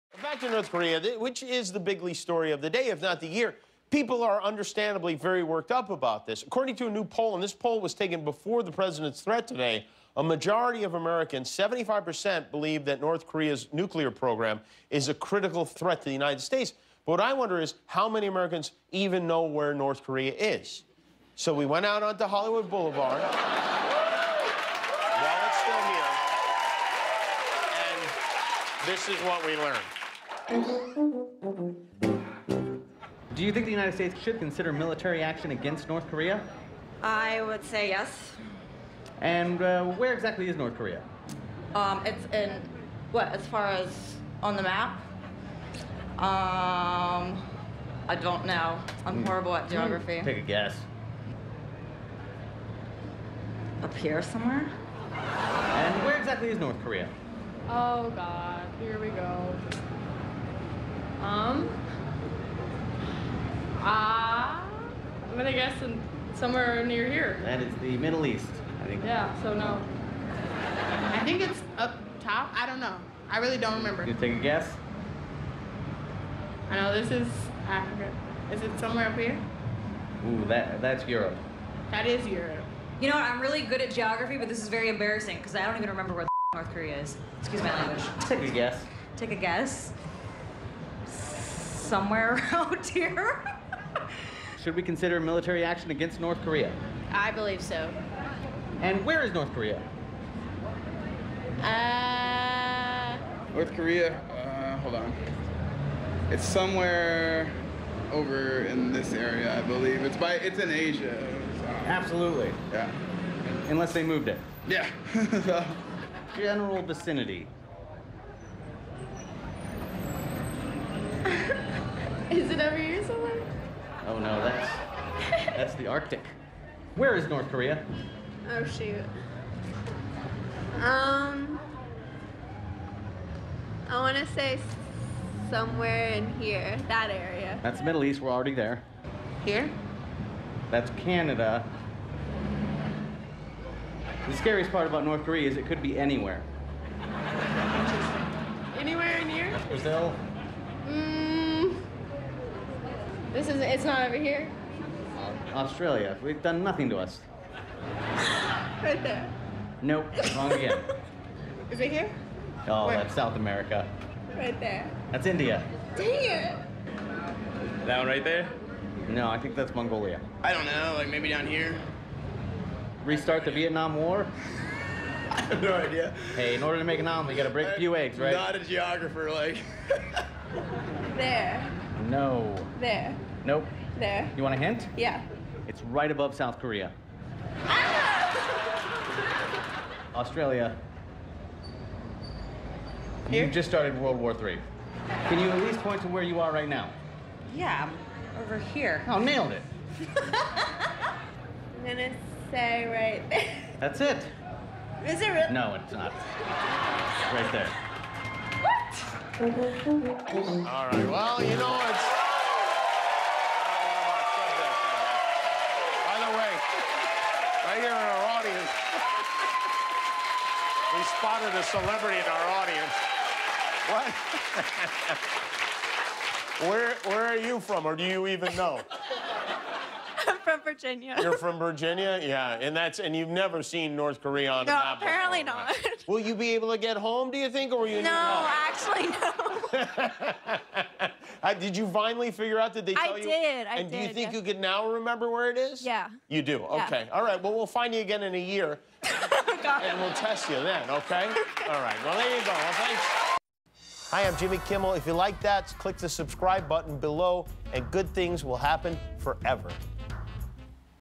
How Many Americans Know Where North Korea Is?
According to a new poll (taken before Trump's 'Fire and Fury' threat) 75% of Americans believe that North Korea's nuclear program is a 'critical threat' to the U.S. This made Jimmy wonder how many Americans even know where North Korea is so we went out to Hollywood Boulevard to find out.